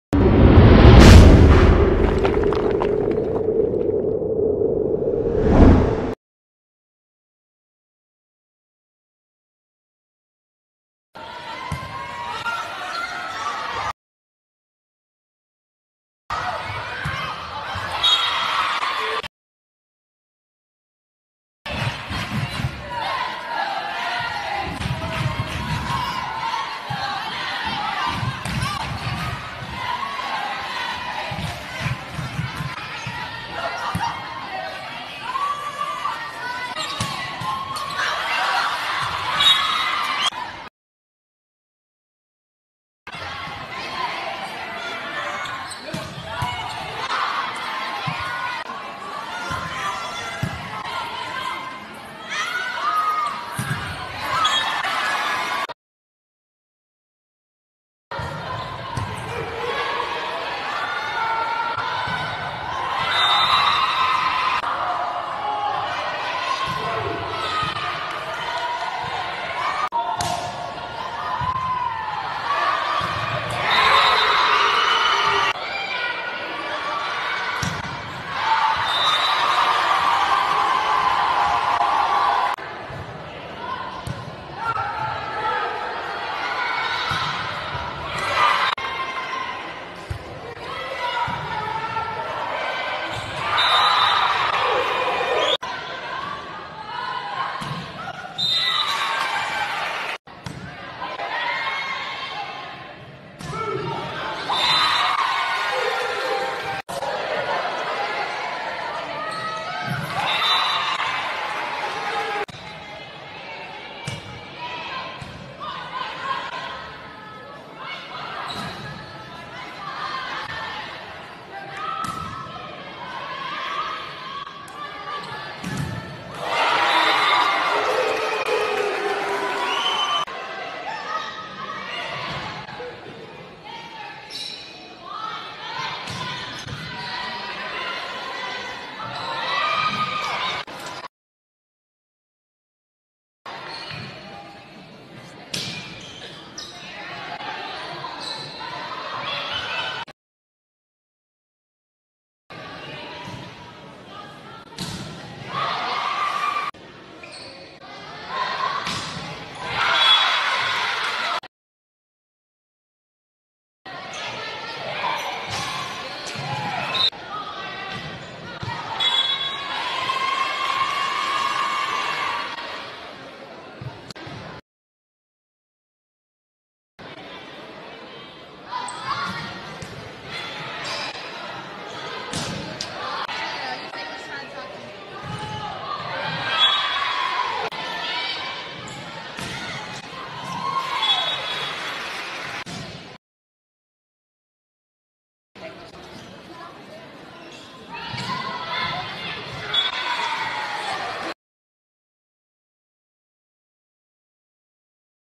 Gabriella Soto 2020 Setter/MH/RS
2018-High School Game Footage

ATHLETE
Setter/Rightside/MH/OH
Major: Kinesology/Business
Height: 6'0" | Weight: 180
Approach Hit: 9'4" | Blocking: 9'2"
Dominant Hand: Right
HIGH SCHOOL
Jersey #30
High School: South San High School
GPA: 4.0
SAT Score: pending
PARENTS
Parent(s) Name: Michael Perez & Veronica Soto
CLUB
Jersey #30
Club: SA Warriors
Coach: Blanca Cortez